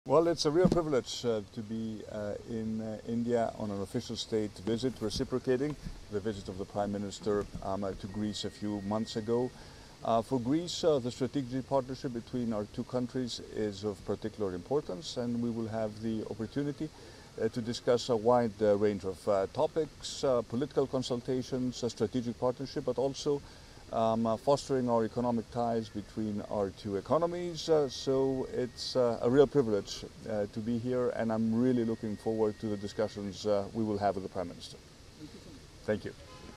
Δήλωση Κυριάκου Μητσοτάκη κατά την επίσημη υποδοχή του από τον Πρωθυπουργό της Ινδίας Narendra Modi
Δήλωση του Πρωθυπουργού Κυριάκου Μητσοτάκη κατά την επίσημη υποδοχή του από τον Πρωθυπουργό της Ινδίας Narendra Modi στο Νέο Δελχί.